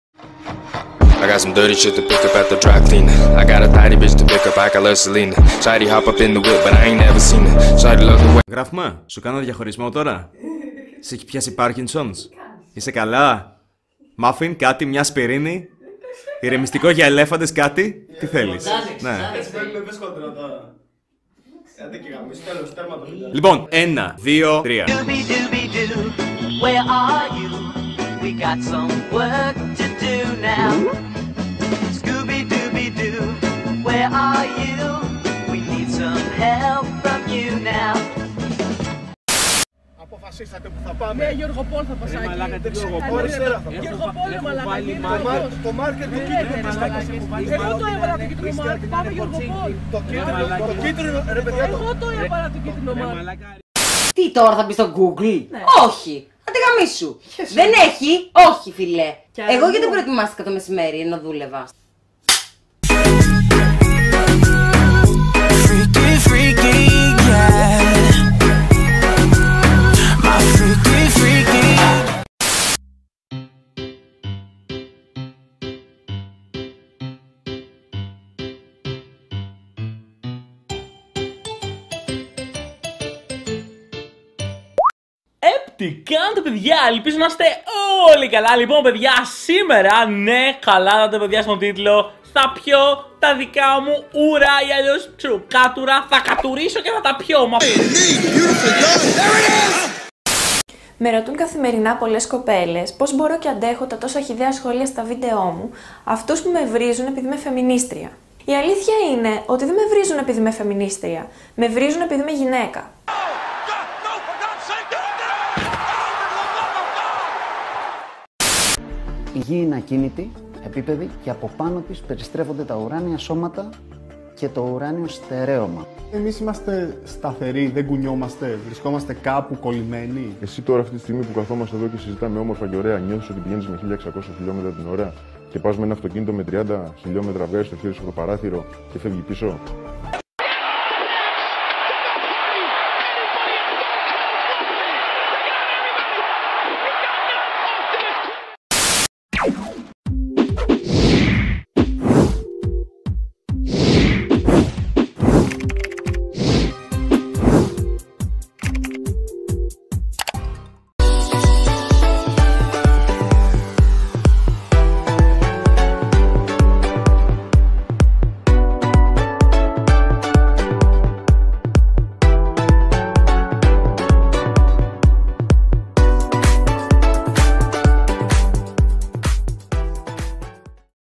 Άναρχο Βίντεο Γνωριμίας #1
Καλημέρα και καλή καλημερΓΕΙΑΑΑΑΑΑΑΑΑ
Σήμερα σε αυτό το βίντεο θα μπούμε στο περίεργο μυαλό μου,
Το youtube μου φαίνεται περίεργο οπότε αντιδρώ και εγώ περίεργα.
Οπότε το άφησα και άνοιξα την τηλεόραση
και αυτά μας δείχνει η ελληνική τηλεόραση μας.